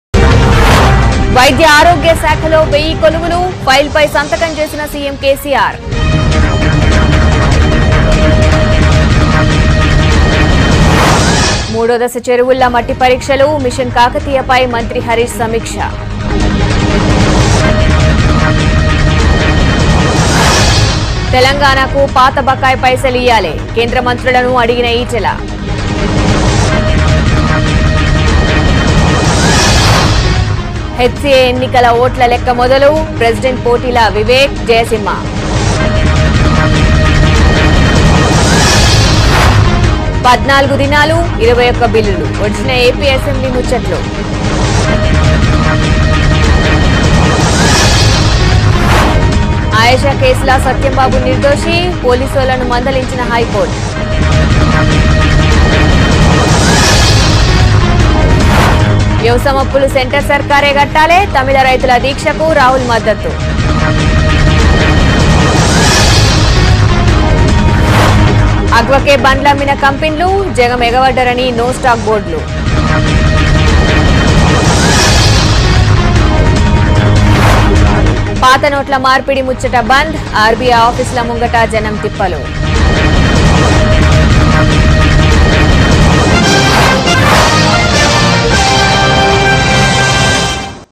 9PM Headlines | HCA Election Counting Begins | AP Assembly Concludes | Ayesha Murder Case | V6 News
Watch V6 News 9PM Headlines - HCA Election Counting Begins | AP Assembly Concludes | Ayesha Murder Case.